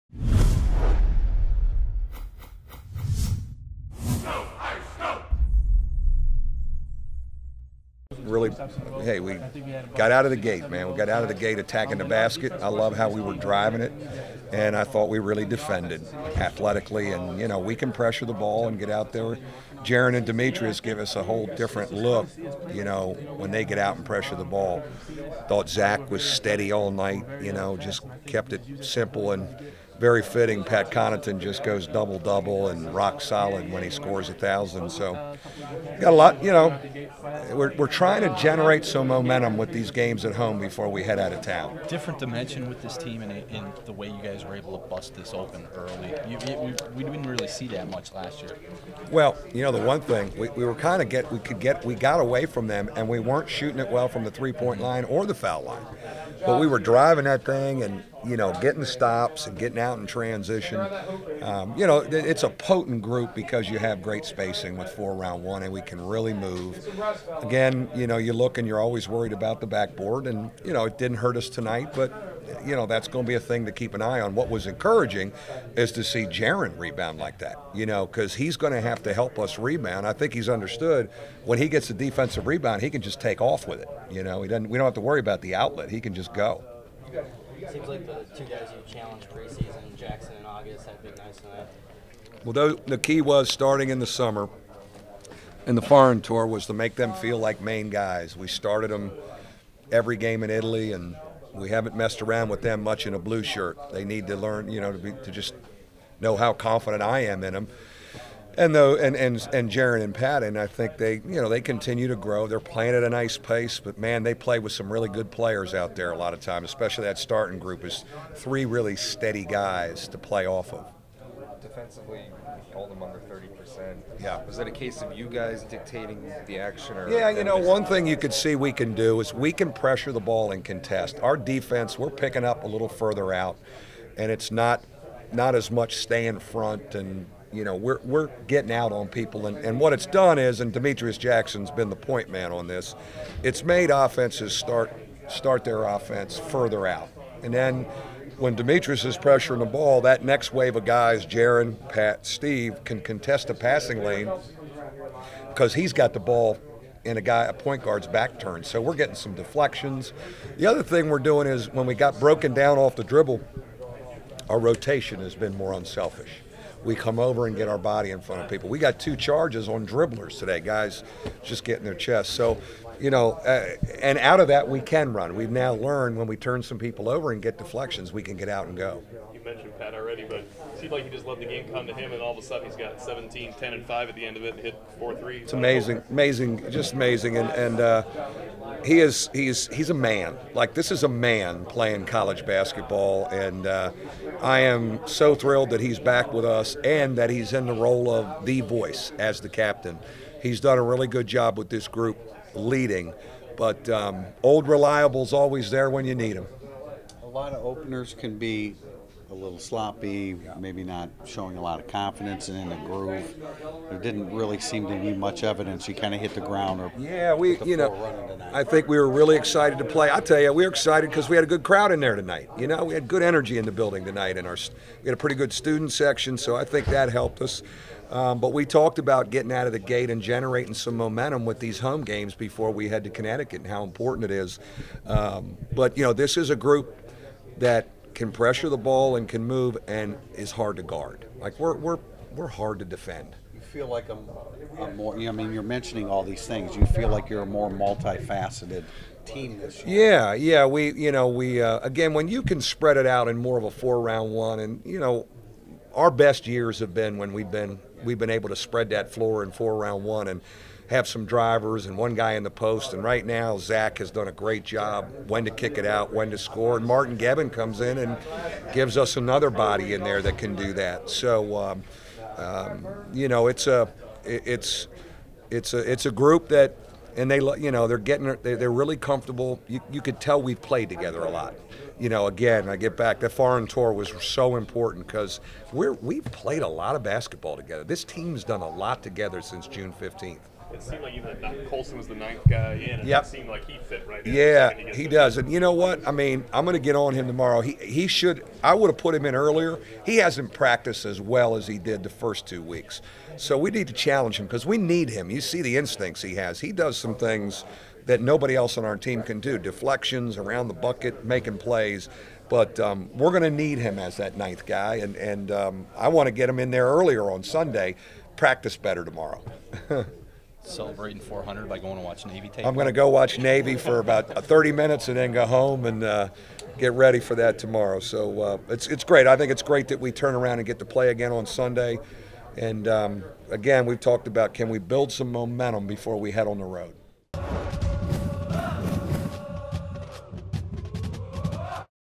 MBB - Coach Brey Binghamton Post Game Media Session
Coach Brey met with the media to discuss Notre Dame's 82-39 win against Binghamton.